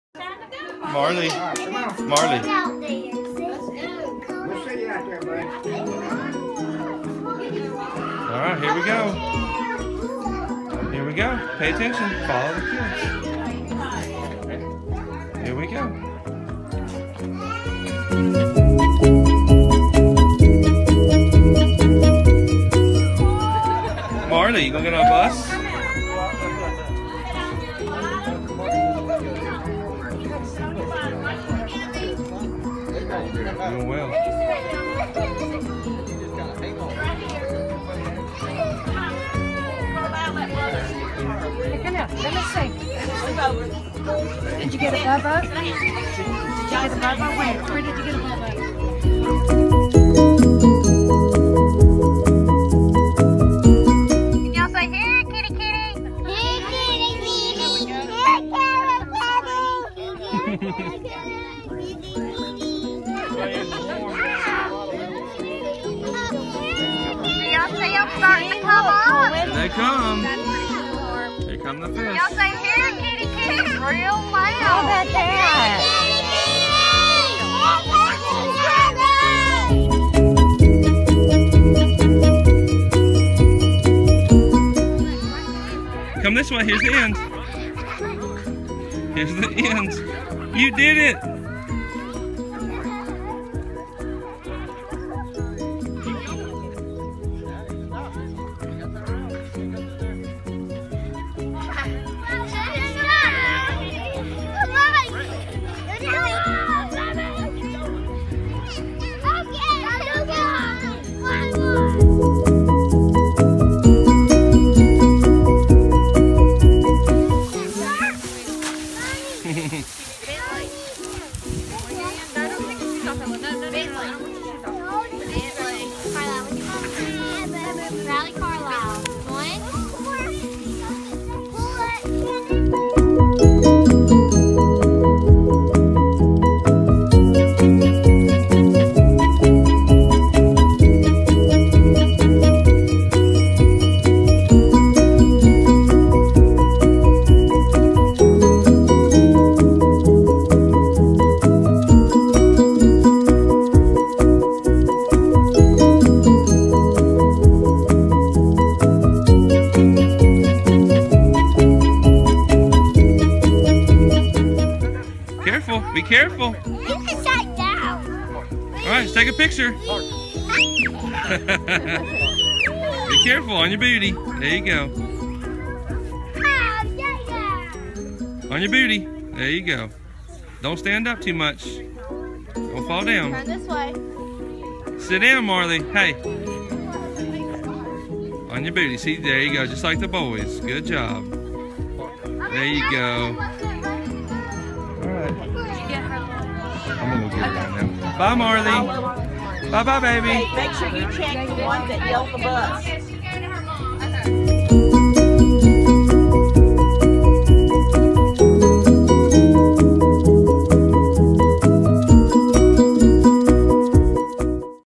Marley's 1st Field Trip
Field trip to the pumpkin patch October 2014